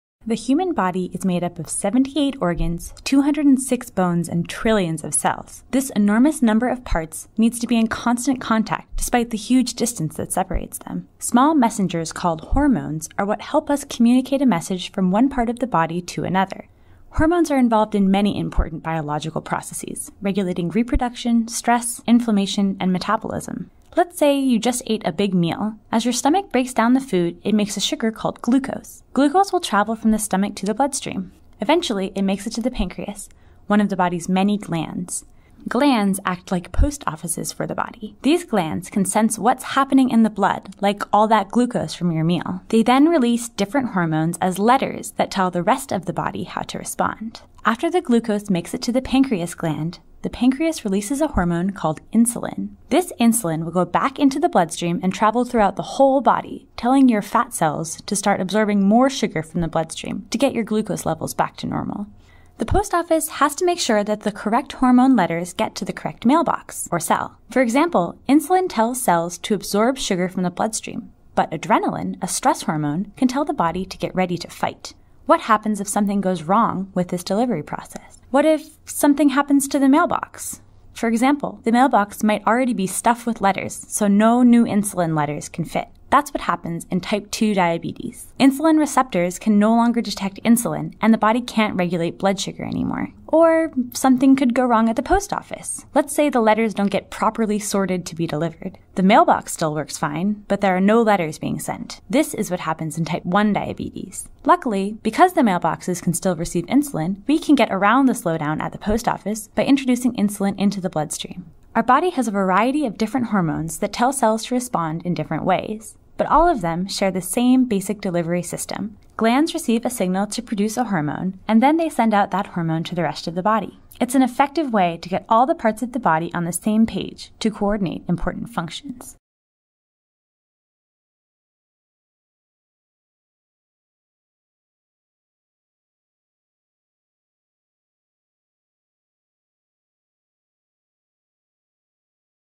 What are Hormones?
What's a hormone? Hormones are messengers that communicate signals from one part of the body to another to control reproduction, stress, metabolism, and inflammation.  This video provides an example of how insulin controls glucose and explains the disease of diabetes. To explore more biology terms and concepts, see the Bio-Dictionary in The Explorer’s Guide to Biology